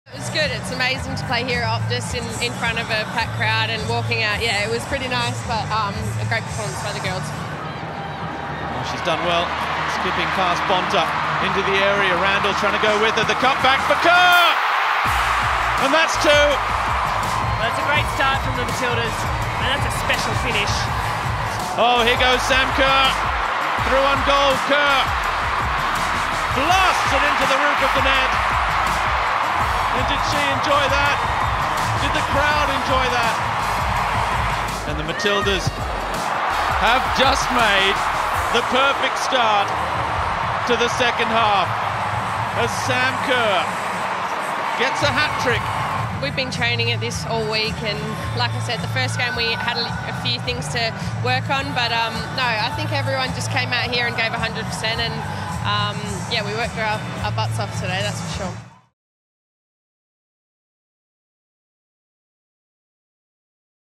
Sam Kerr looks back at her hat-trick and the Matildas' dominant performance
After scoring a hat-trick in front of her hometown crowd, Sam Kerr reflects on the side's dominant performance in their 8-0 victory over the Philippines.